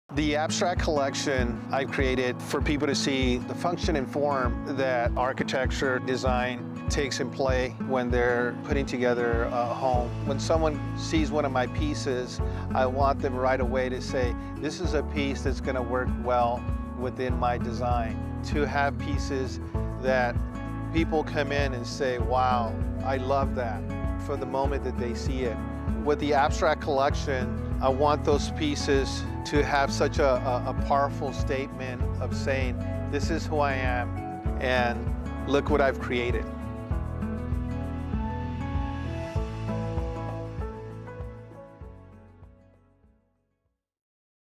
Fine Art Canvas | Abstract Collection by Karlos Marquez | FineArtCanvas.com
Karlos Marquez presents his abstract collection, available on Fine Art Canvas. This unique array of statement pieces features bold architectural compositions that Marquez designed to work with your decor designs.

►Follow FAC for updates on new artists, canvas collections, and more!